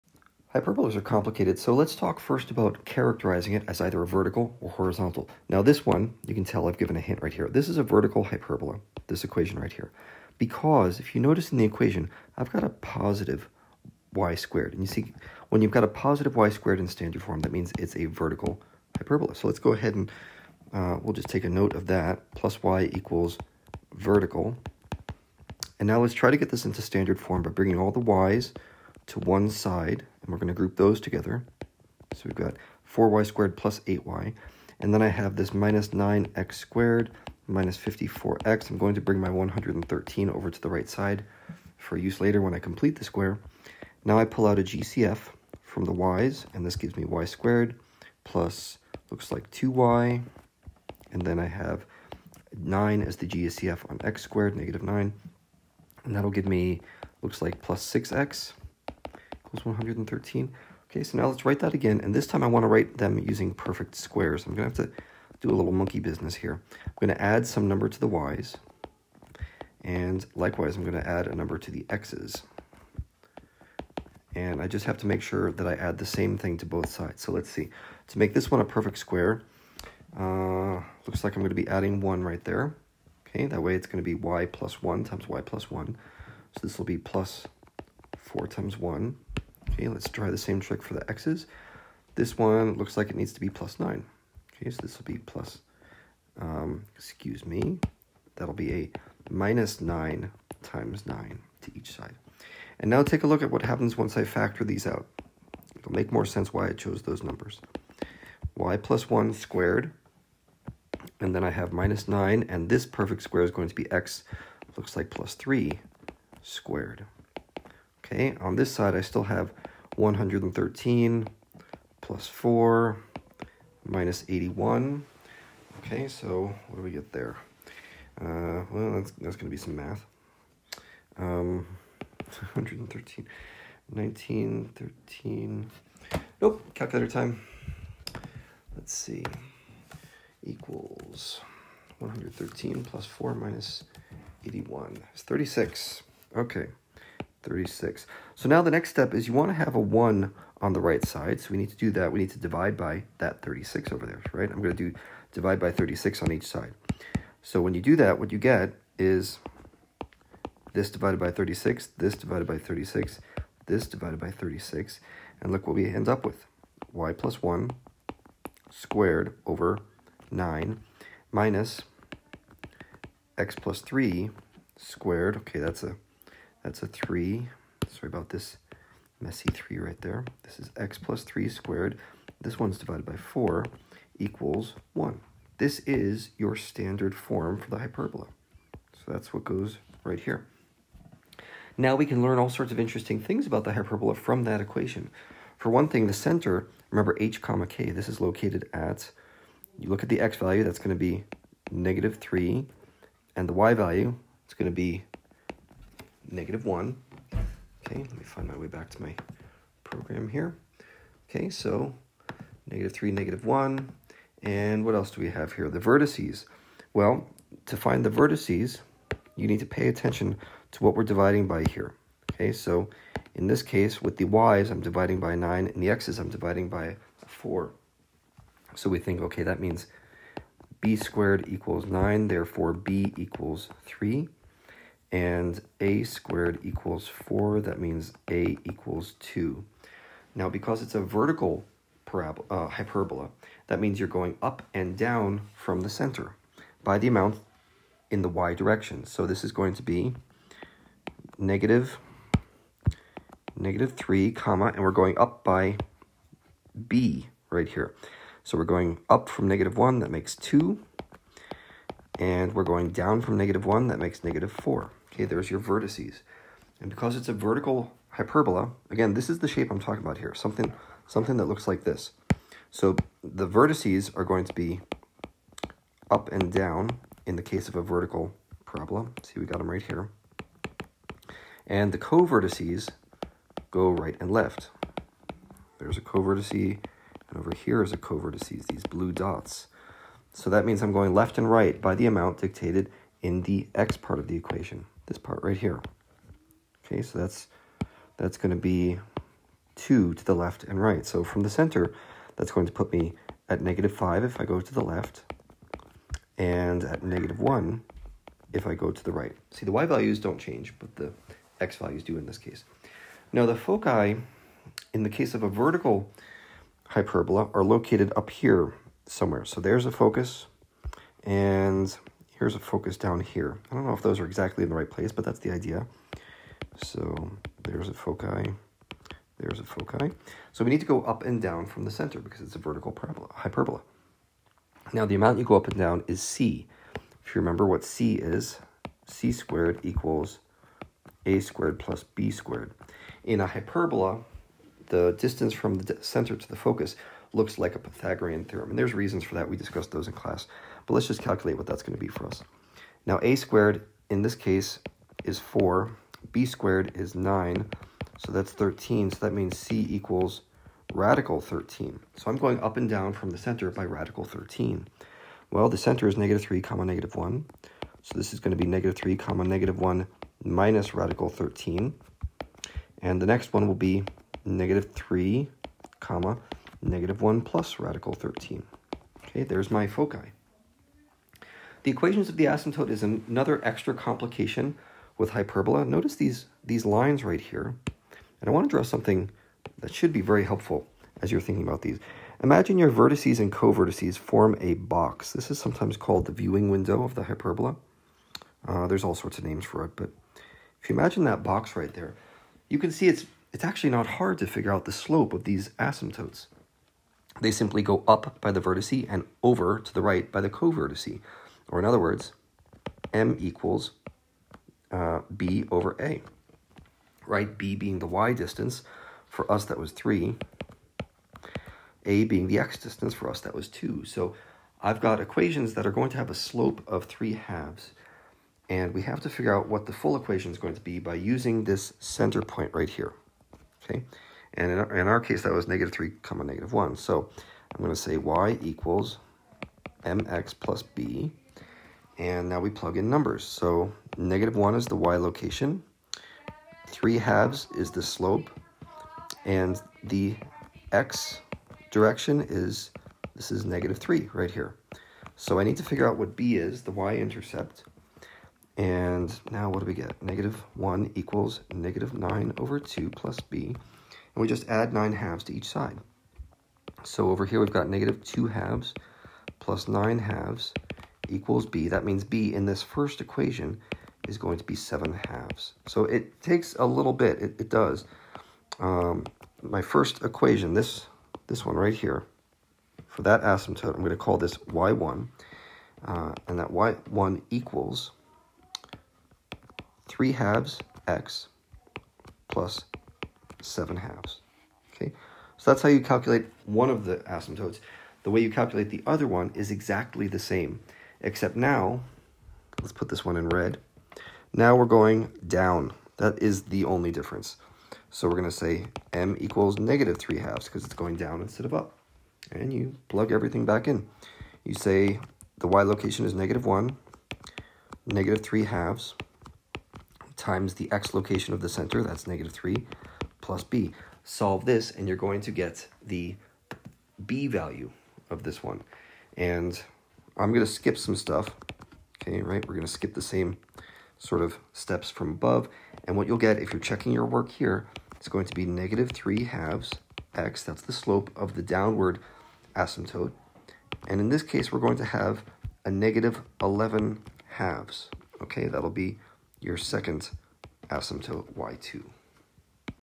CE1050.05.50 — Characterize Vertical Hyperbola by its Equation
Murphy Math --
Characterize a vertical hyperbola using its equation in non-standard form.  Includes discussion of how to characterize a hyperbola as vertical vs horizontal, and how to rewrite the equation of a hyperbola in standard form.  Determine location of vertices, co-vertices, center, foci, and the equations of the asymptotes.